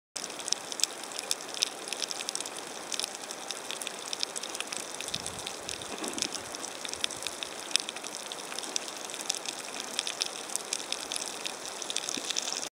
Leopard Flounder, Bothus pantherinus
A Leopard Flounder at a depth of 5 m, Waikiki Beach, Honolulu, Hawaii, 7th December 2013.